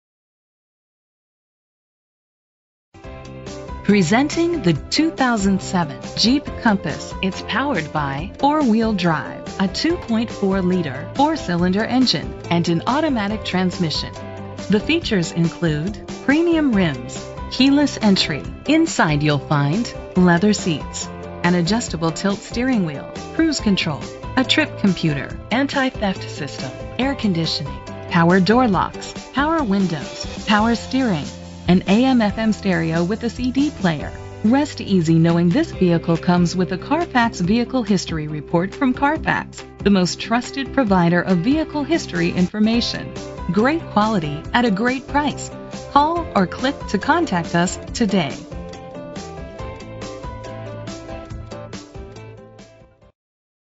2007 Jeep Compass 71658A - Sandy UT
Phone:
Year: 2007
Make: Jeep
Model: Compass
Trim: Limited
Engine: 2.4 liter 4 cylinder 4WD
Transmission: Automatic
Color: Black
Mileage: 113495
Address:
VIN:1J8FF57W77D143944